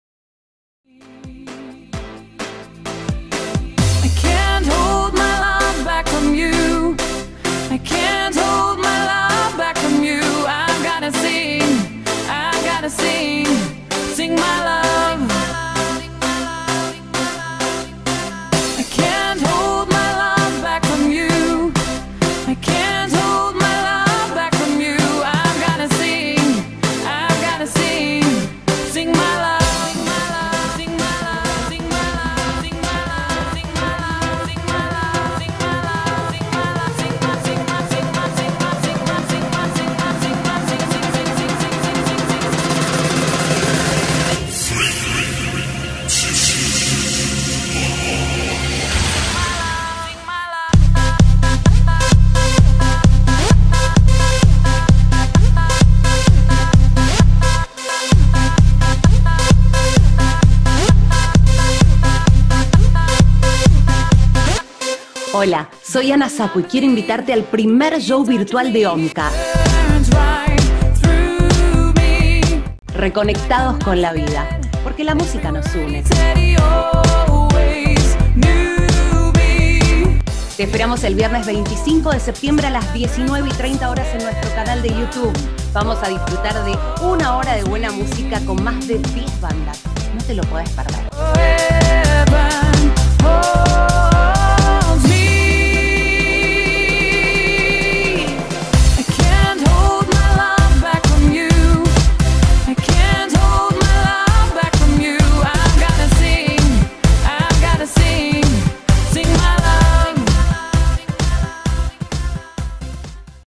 Festival de la Primavera Virtual
En este tiempo de aislamiento social obligatorio en el que todavía no se permiten eventos públicos, la Organización de Músicos Cristianos Argentinos realizará este festival en forma virtual con participantes de todo el país. ¡No te lo pierdas!